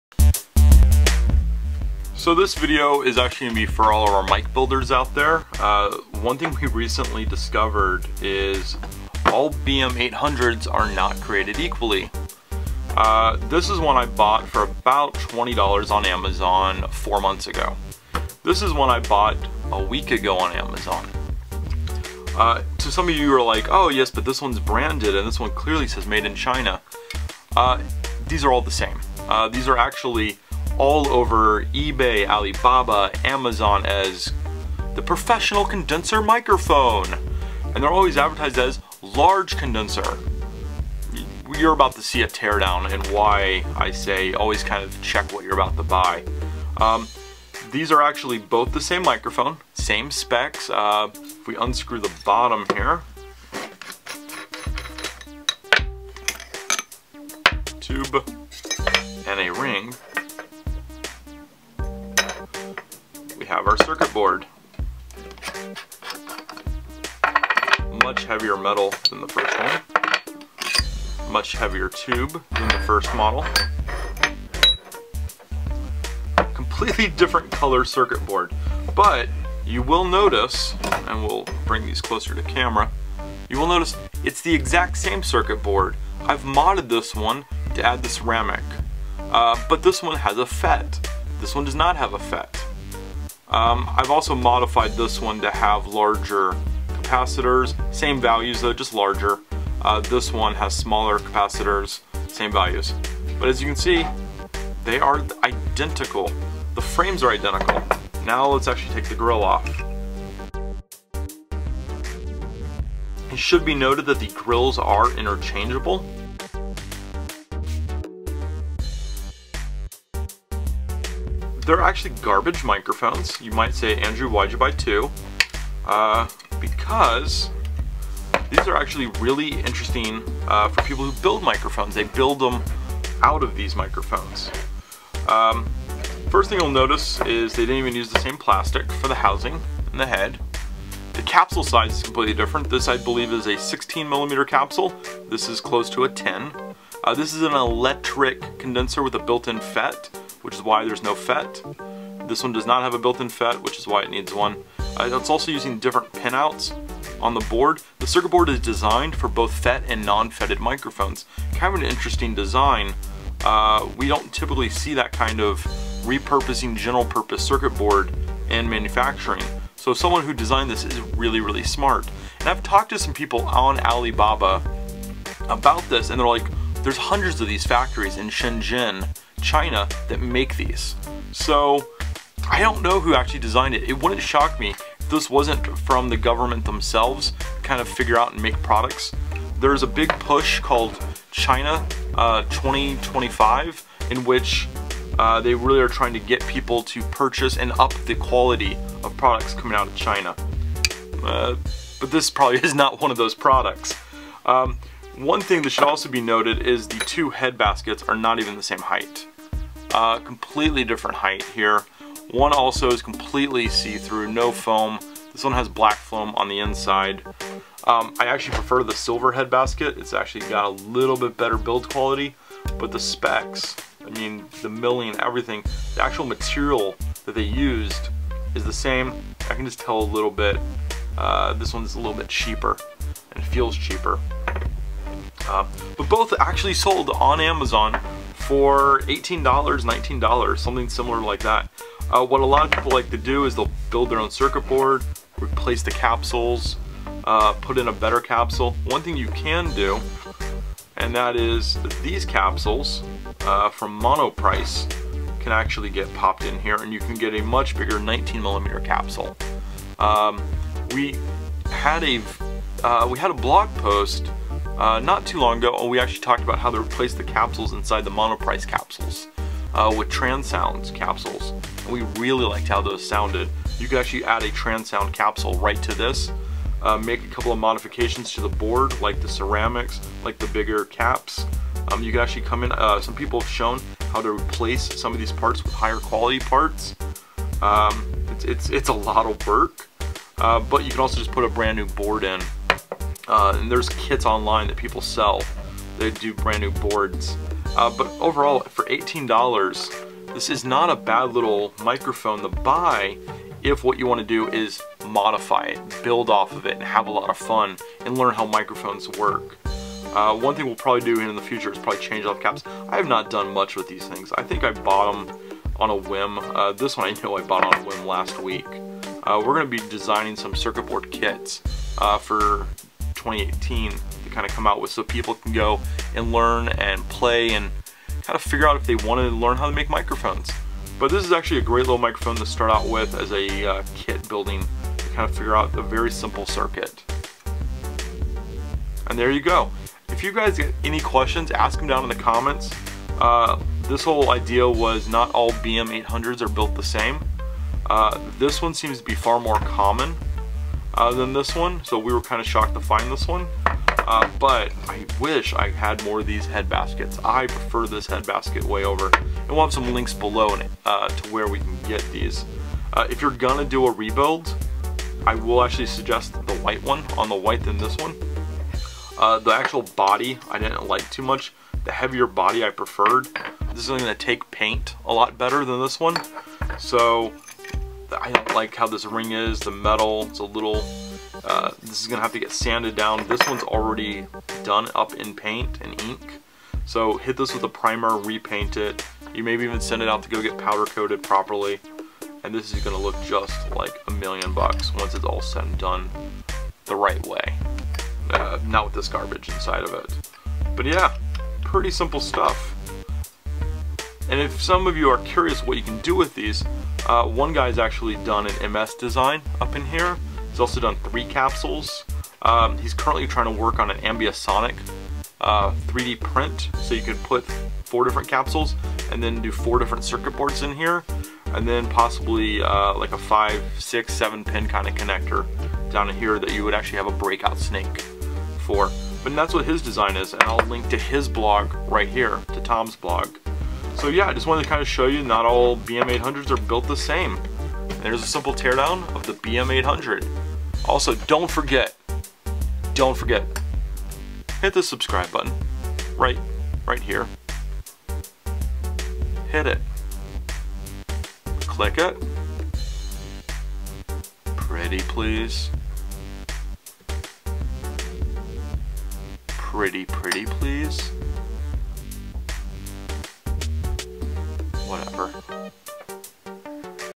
BM-800 Microphone Teardown And Comparison
I break down the different model types of the BM-800 microphone that is so common on eBay, Amazon, and AliExpress.

Here is the Amazon Link to the White BM800 with the better breadbasket and stronger metal body.

If you want the full BM800 package here is one that I have purchased before.